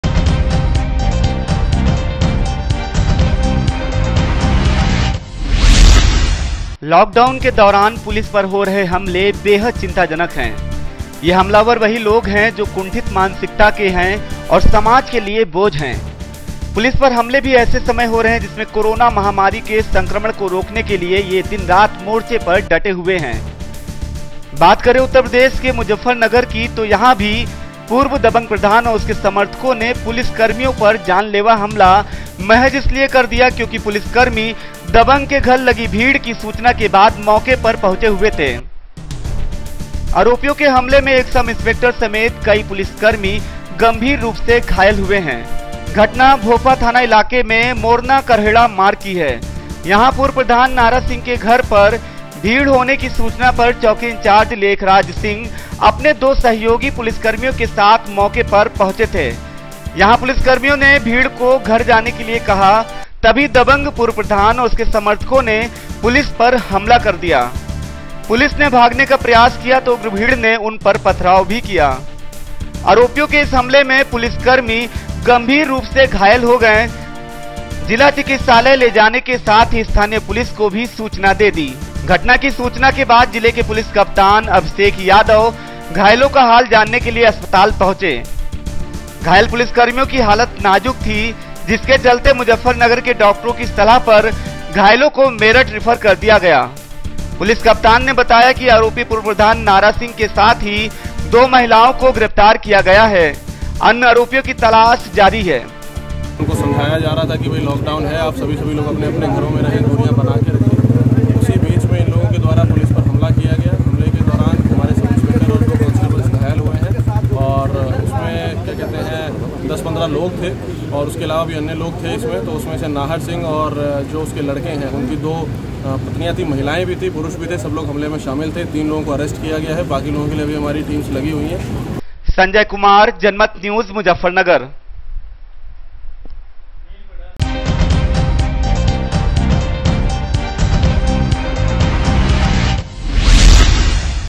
भीड़ ने पुलिस पर किया हमला, कई पुलिस कर्मी हुए घायल
लॉक डाउन के दौरान पुलिस पर हो रहे हमले बेहद चिंताजनक है। ये हमलावर वही लोग है जो कुंठित मानसिकता के है और समाज के लिए बोझ है। पुलिस पर हमले भी ऐसे समय पर हो रहे है जिसमे कोरोना महामारी के संक्रमण को रोकने के लिए ये दिन - रात मोर्चे पर डटे हुए है। बात करे उत्तर प्रदेश के मुजफ्फरनगर की तो यहाँ भी पूर्व दबंग प्रधान और उसके समर्थकों ने पुलिस कर्मियों पर जानलेवा हमला महज इसलिए कर दिया क्योकि पुलिस कर्मी दबंग के घर लगी भीड़ की सूचना के बाद मौके पर पहुंचे हुए थे।News in Hindi (Janmat News) :JANMAT janmatnews_BREAKING Breaking_News UP Special | Morning_News | Watch video